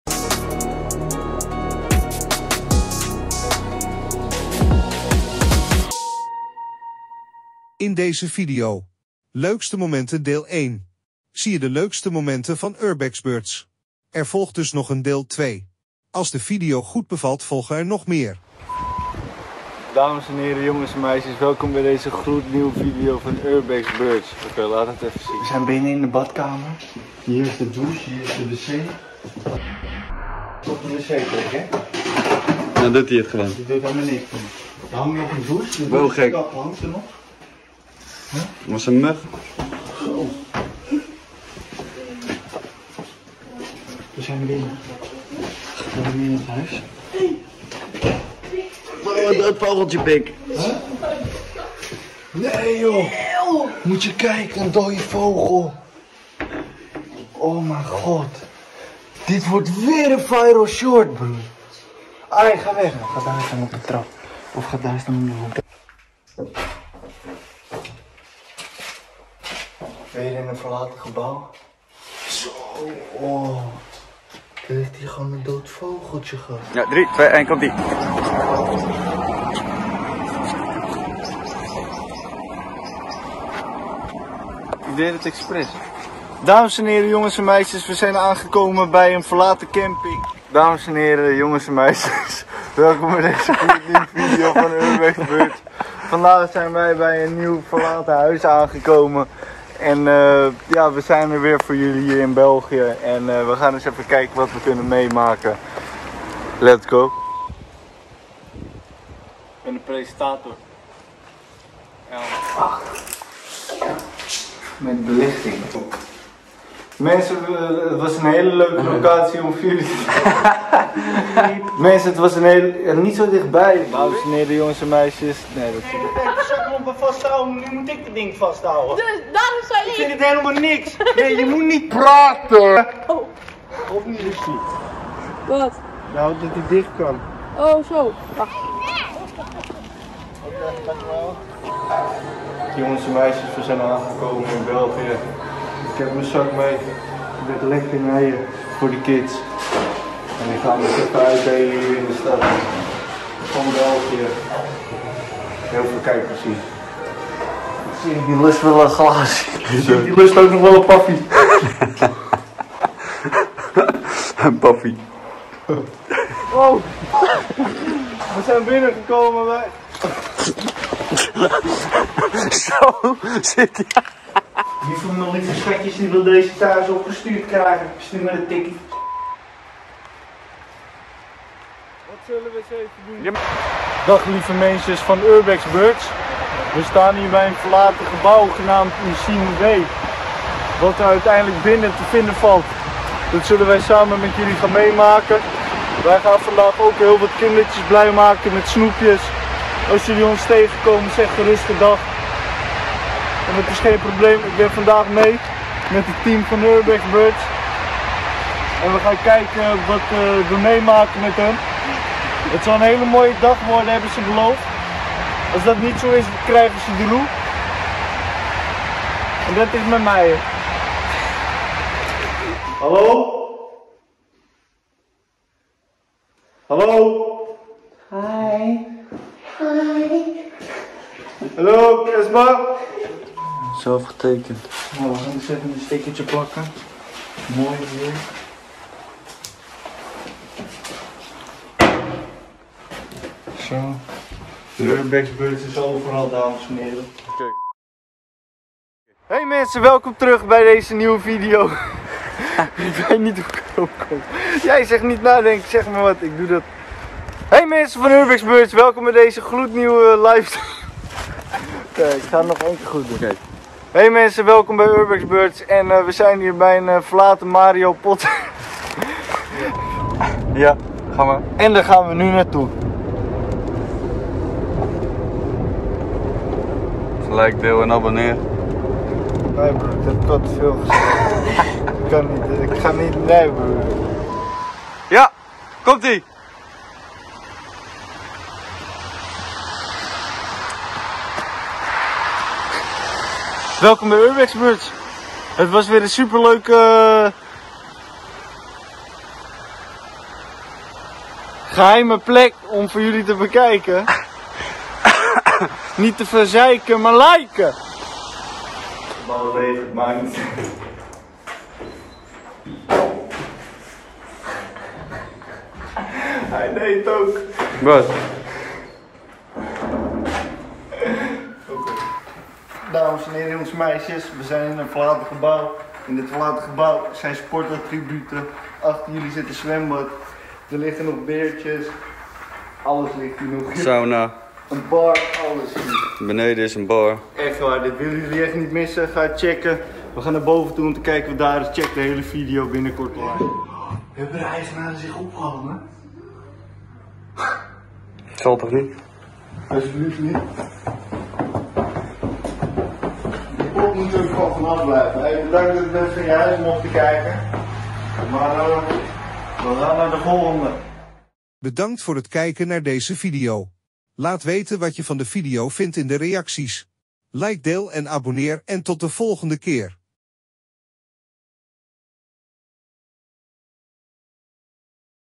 LEUKSTE MOMENTEN URBEXBIRDS DEEL #1
Welkom bij "Urbexbirs Leukste Momenten Deel 1"! In deze video nemen we je mee op een avontuurlijke reis door enkele van de meest fascinerende en mysterieuze urbex-locaties die we hebben bezocht. Van verlaten fabrieken tot spookachtige ziekenhuizen, elk moment zit vol spanning en ontdekking.